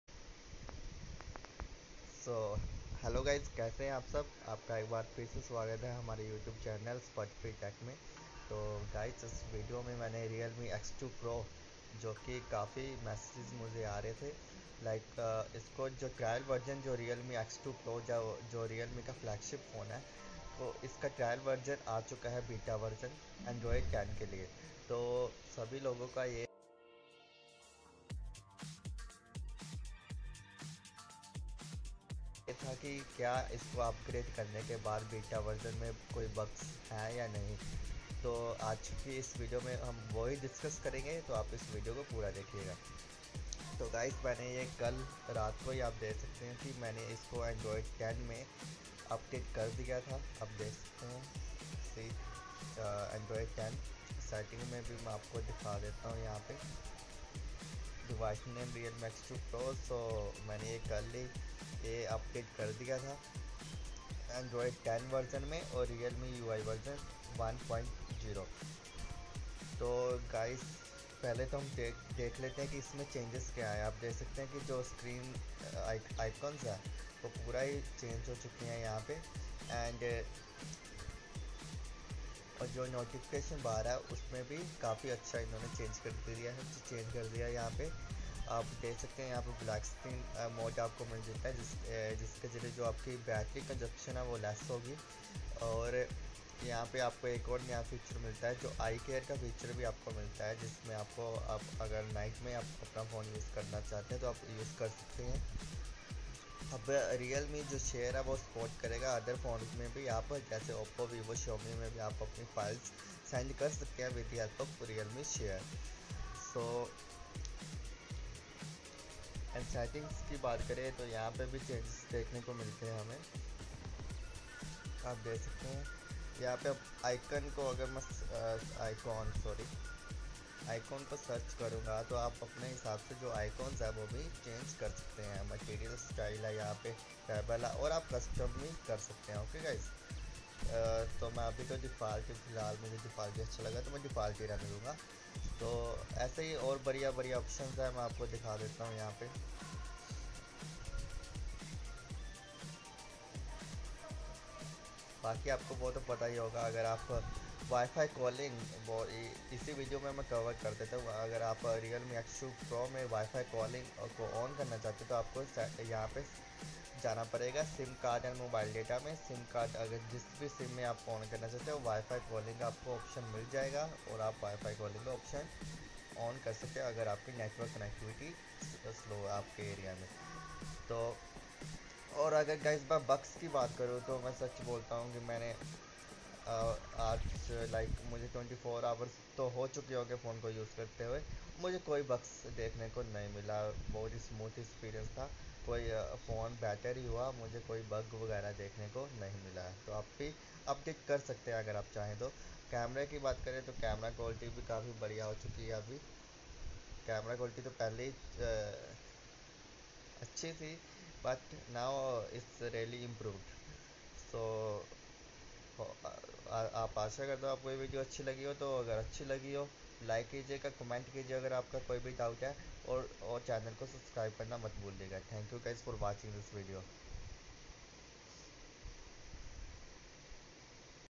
Real Me X2 Pro Beta Android 10 Upgraded!
Hello, guys

In this video i will clear your doubts about Android 10 beta upgrade in x2 pro.

Jai Hind!